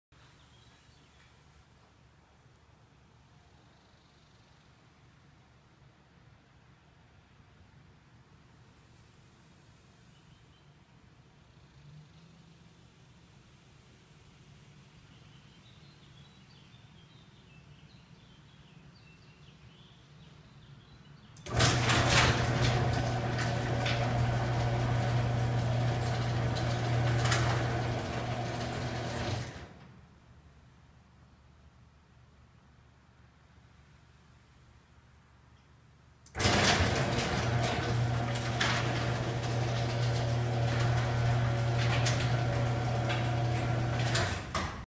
DIY Garage door opener from iPhone
This is a simple demo using the NodeMCU (based on the ESP8266 chip).
It's running nodemcu-httpserver and one of the built-in demos which I developed